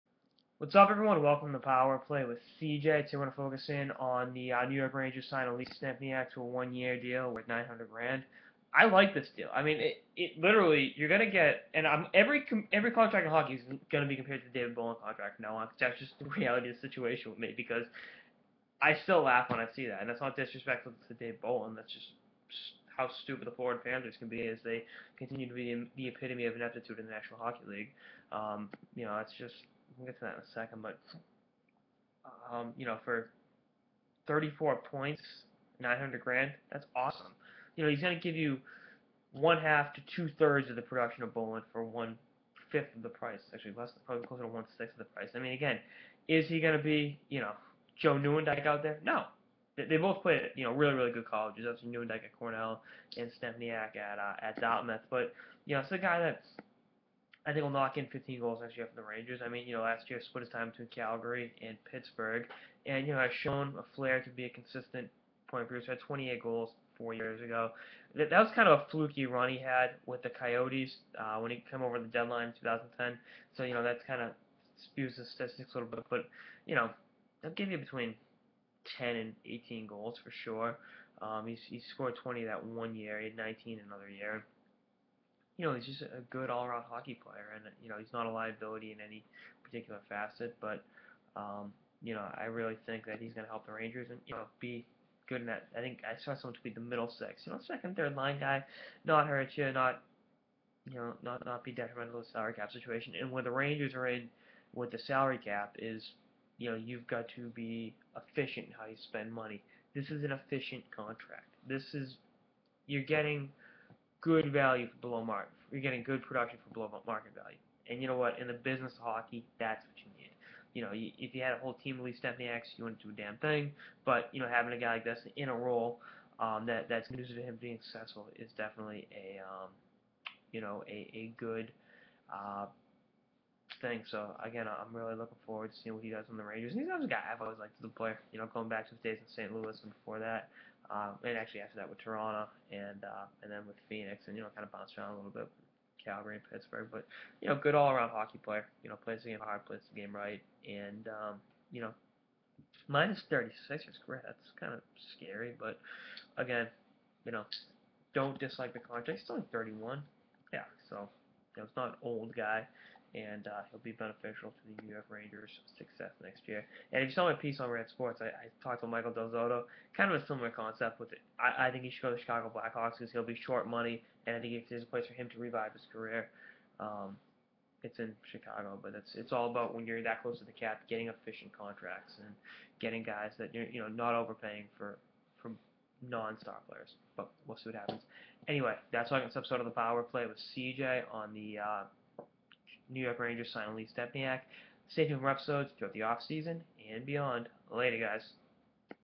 Power Play with CJ: New York Rangers Signing Lee Stempniak
CJ dishes on the Blue Shirts inking Stempniak to a one year deal and why he loves the move.